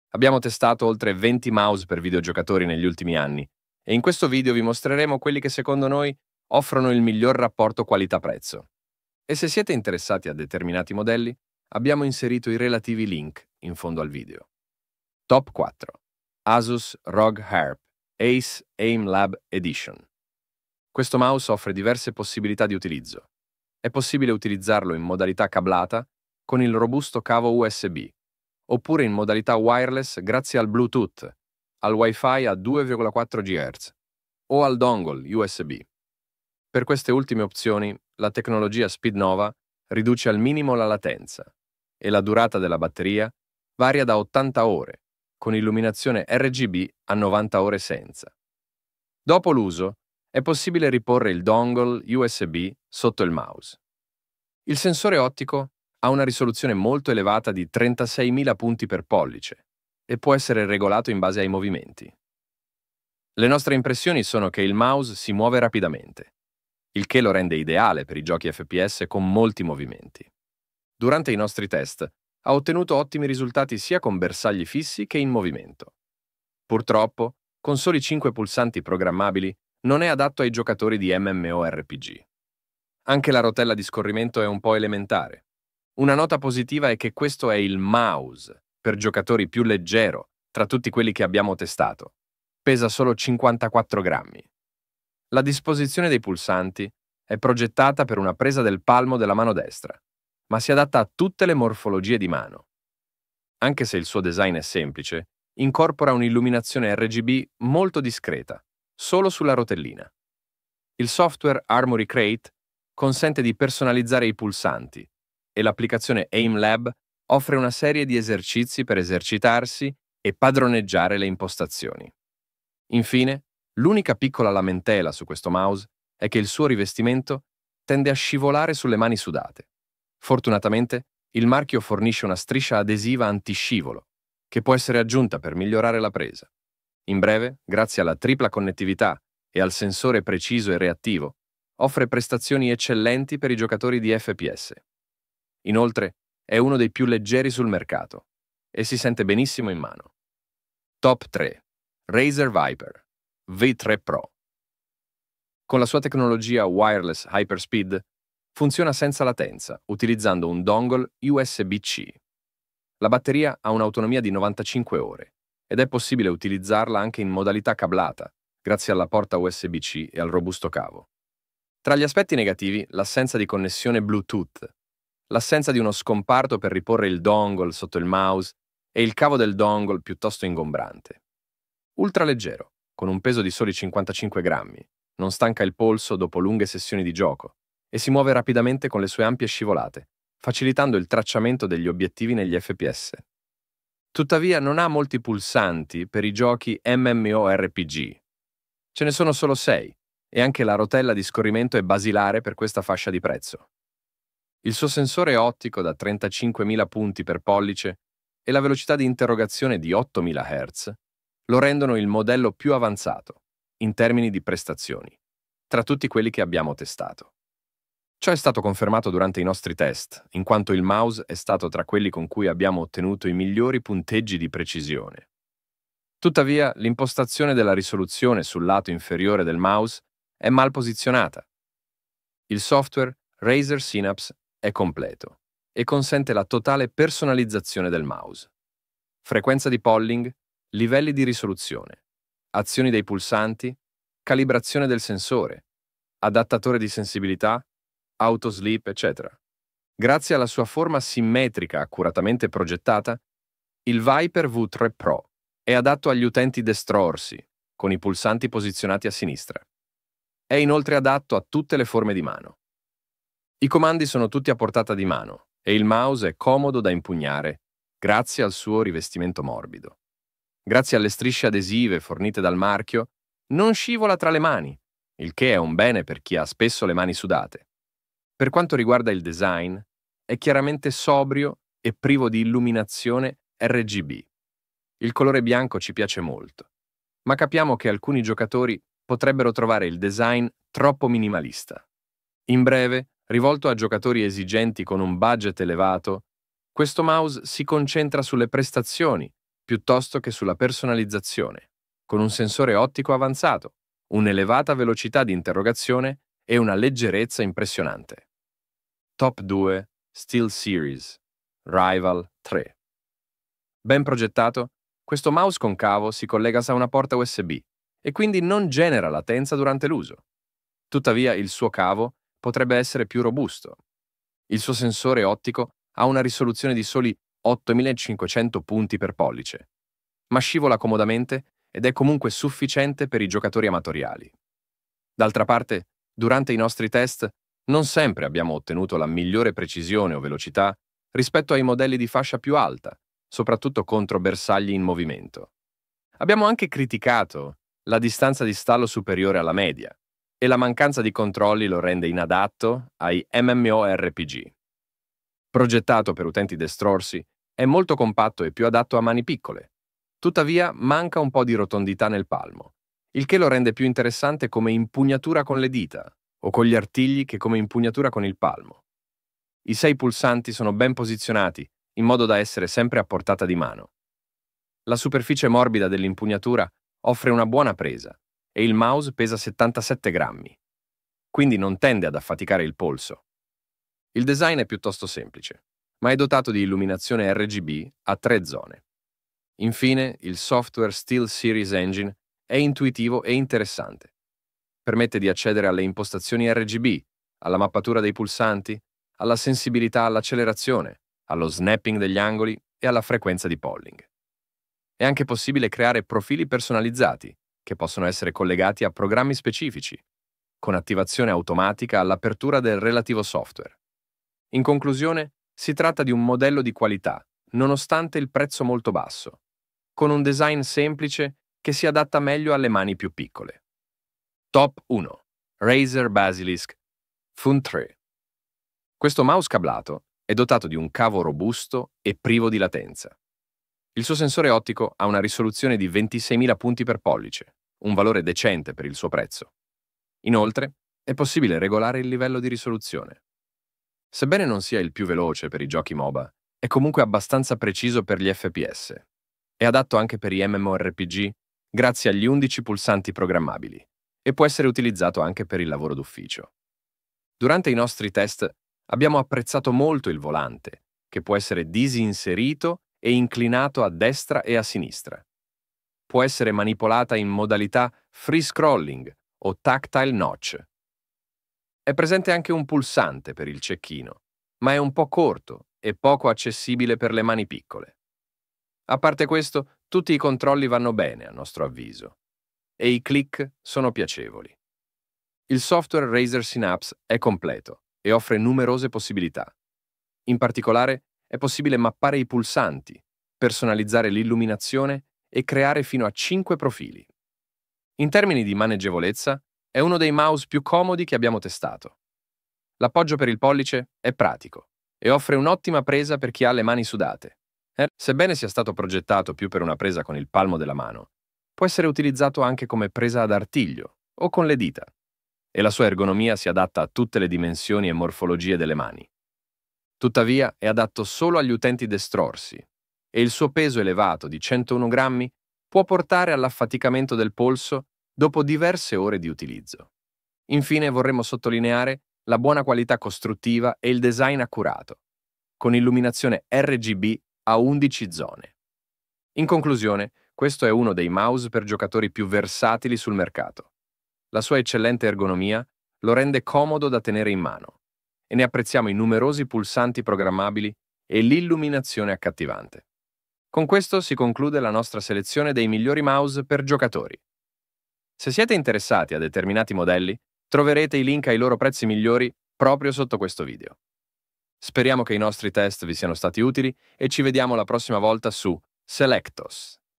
Abbiamo TESTATO i migliori mouse per gaming
Top 4 🟠 Asus ROG Harpe Ace Aim Lab Edition ★
Top 3 🟠 Razer Viper V3 Pro ★
Top 2 🟠 SteelSeries Rival 3 ★
Top 1 🟠 Razer Basilisk V3 ★